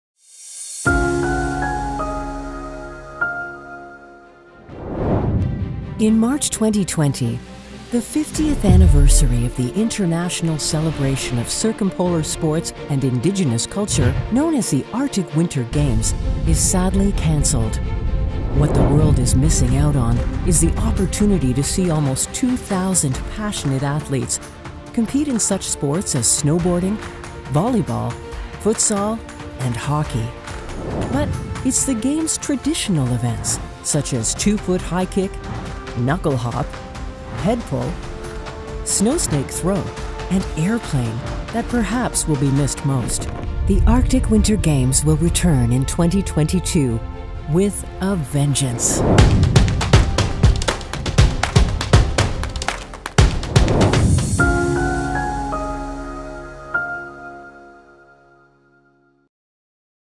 GeoMinute: Celebrating 50 years of the Arctic Winter Games
The 50th anniversary of the immensely popular Arctic Winter Games was unfortunately cancelled due to the COVID-19 pandemic.

Credits:
Logos courtesy Arctic Winter Games International Committee
Athlete photos courtesy Arctic Winter Games 2016 Greenland and Pat Kane Photo

This project has been made possible in part by the Government of Canada.